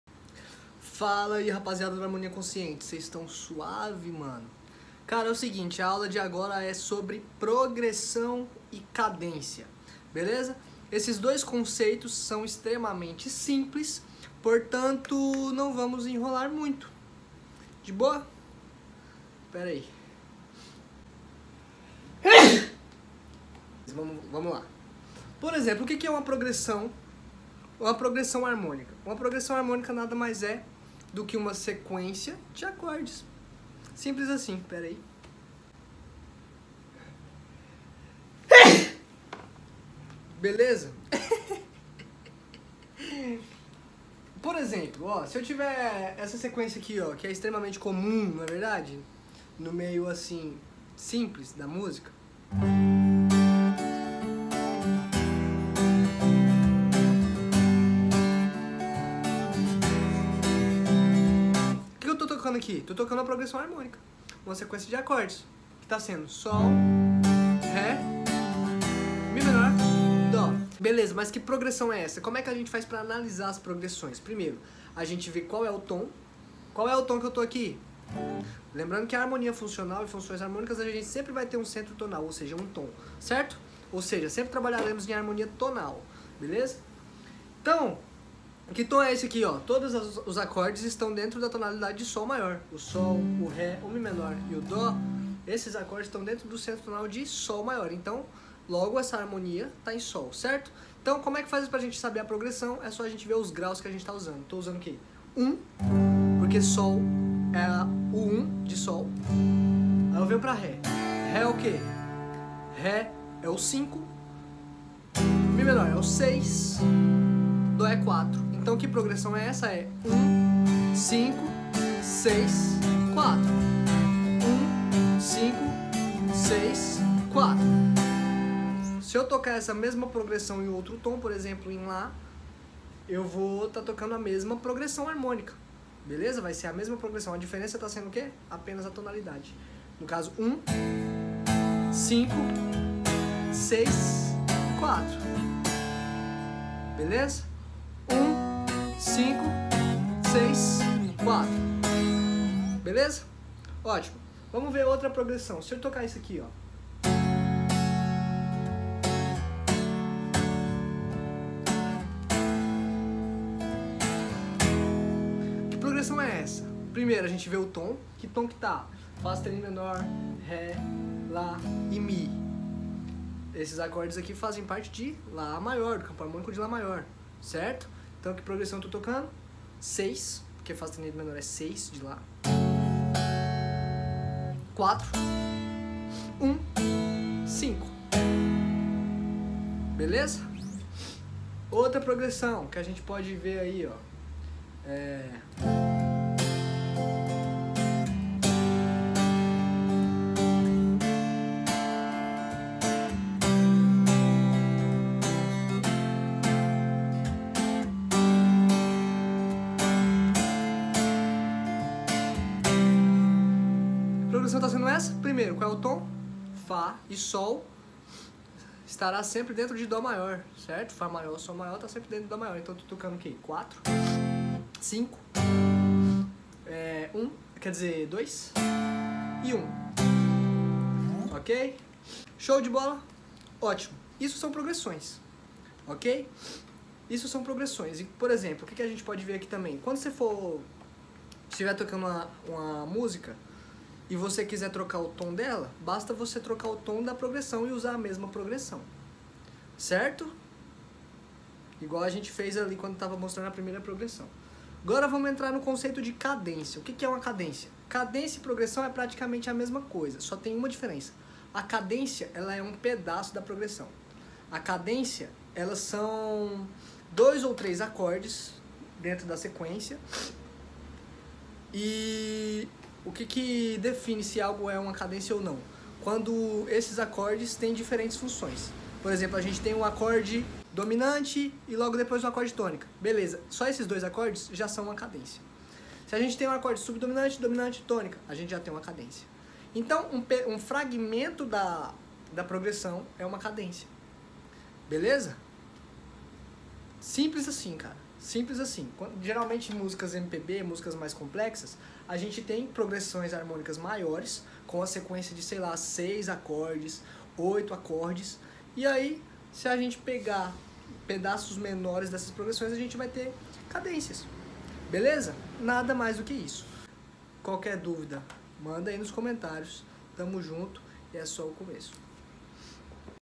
Como mudar o tom da música (FÁCIL e RÁPIDO)
Como modular, ou trocar o tom da música, modulação , em harmonia funcional , pra quem toca guitarra , e violão

Aula tirada do considerado melhor curso de harmonia e teoria musical pra guitarristas o HARMONIA CONSCIENTE